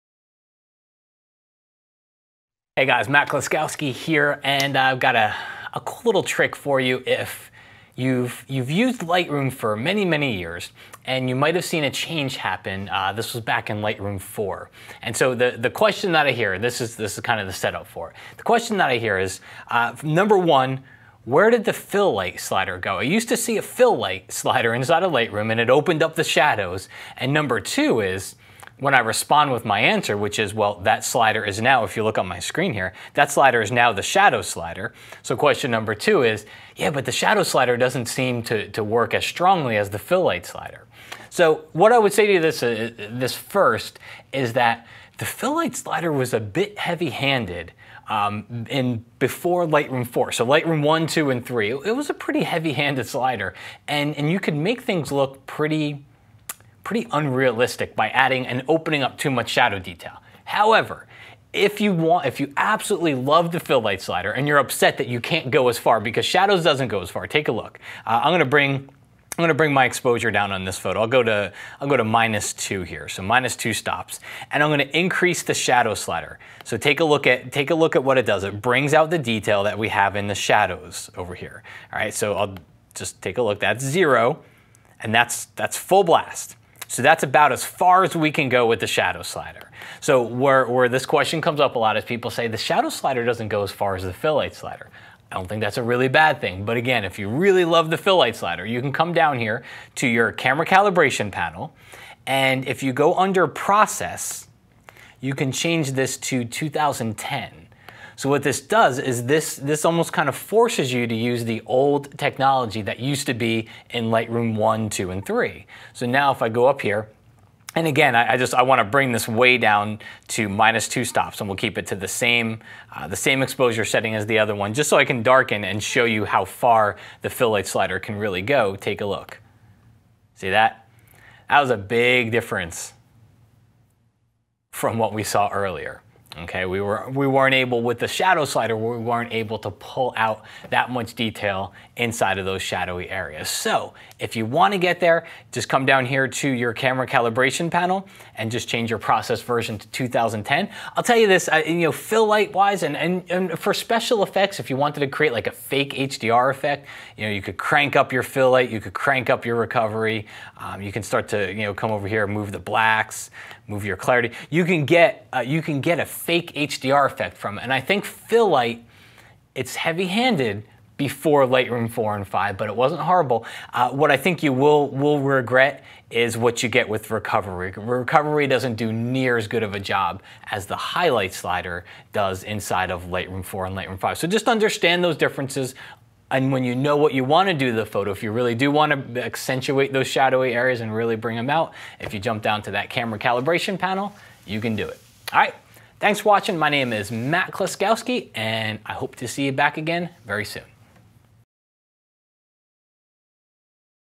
How To Bring Back The Old Fill Light Slider in Lightroom 5
This time around, Matt Kloskowski shows you how to bring back the old Fill Light slider from Lightroom version 1, 2 or 3 (which is used to open up and reveal shadowy areas), and get the same slider in Lightroom 5.